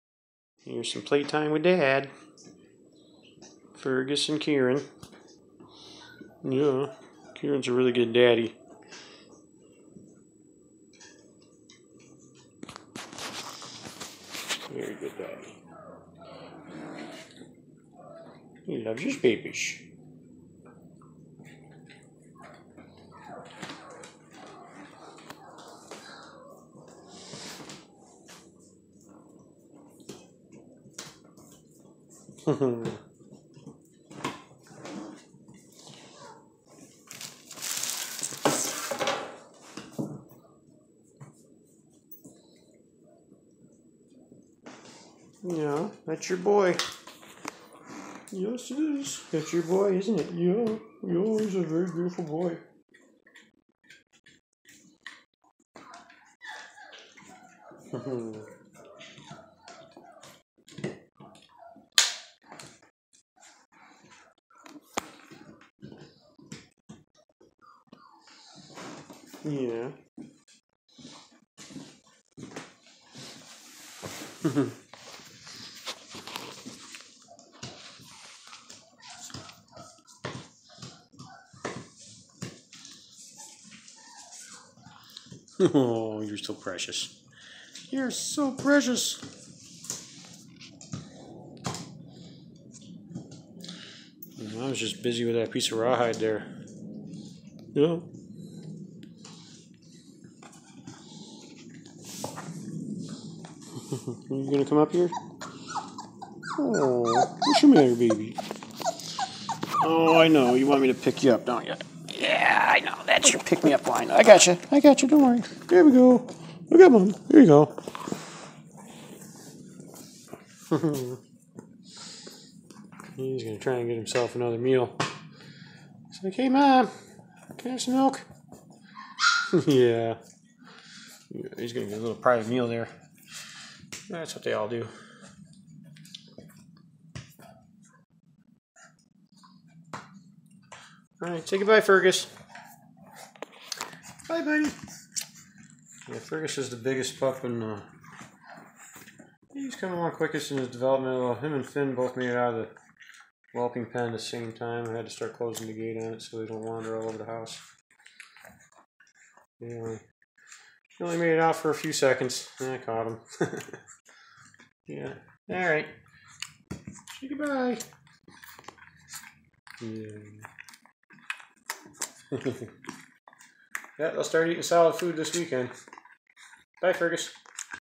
Fergus Playing With His Daddy
Whippet puppy Fergus playing with his daddy Kieran before private nursing session with Mum.